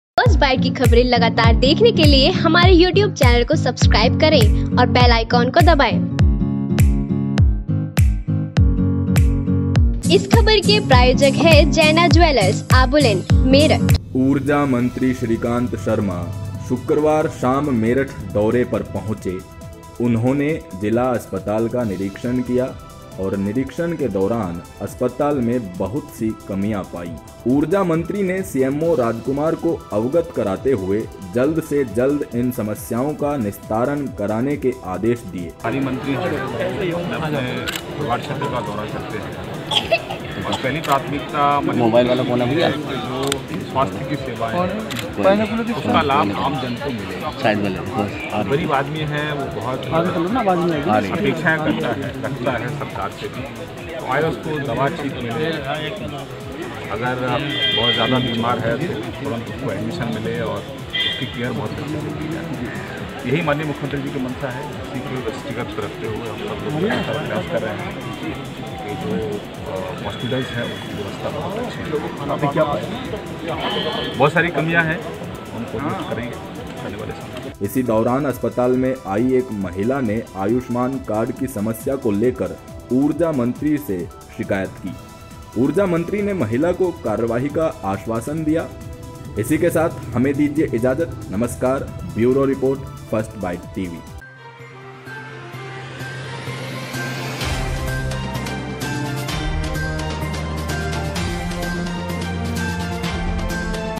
ऊर्जा मंत्री श्रीकांत शर्मा ने किया मेरठ जिला अस्पताल का निरीक्षण
ऊर्जा मंत्री श्रीकांत शर्मा ने किया मेरठ जिला अस्पताल का निरीक्षण और सीएमओ राजकुमार को  समस्या को ठीक करने के आदेश दिए